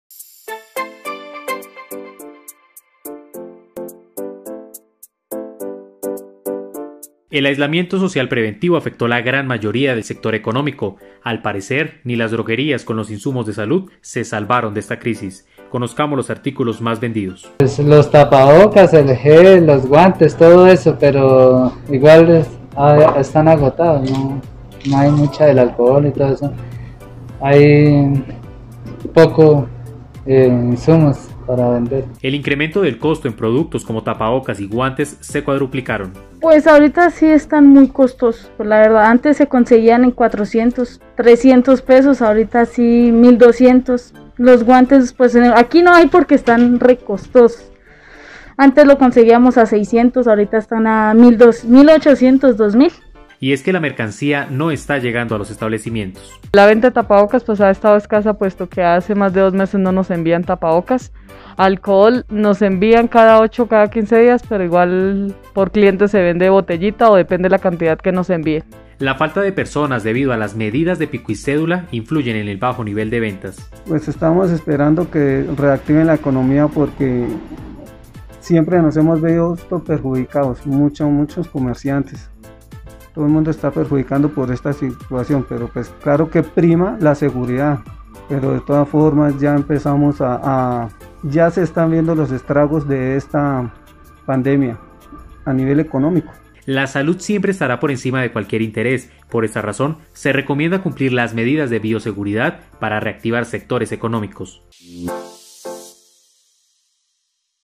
Top 3 - No da tregua esta batalla entre comerciantes y el aislamiento social preventivo
No da tregua esta batalla entre comerciantes y el aislamiento social preventivo